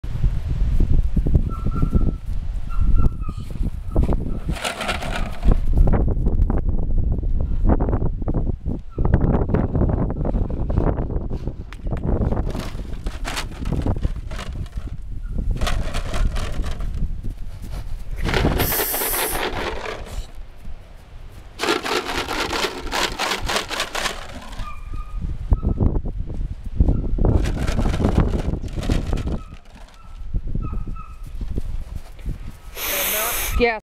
Khalies Joy
Yearling straight egyptian filly by Ali Bayfire x EAI Khalnoreena